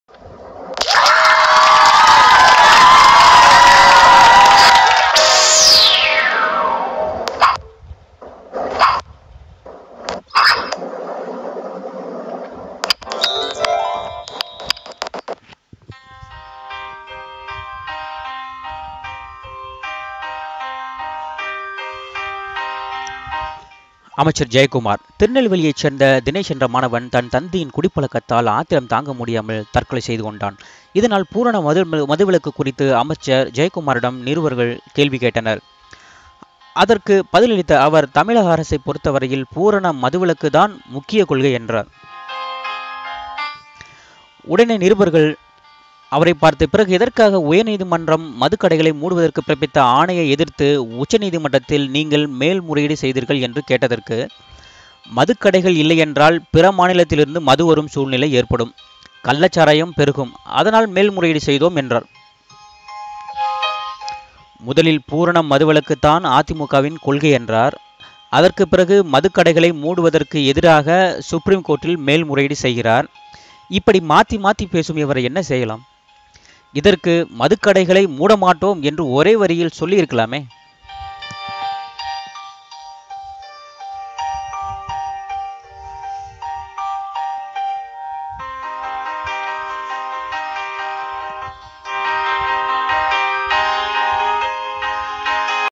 மாத்தி மாத்தி பேசும் அமைச்சர் ஜெயக்குமார் | Minister Jayakumar's DUBAKOOR Speech
Please watch this video which is about ADMK Minister Jayakumar dubakoor speech about TASMAC.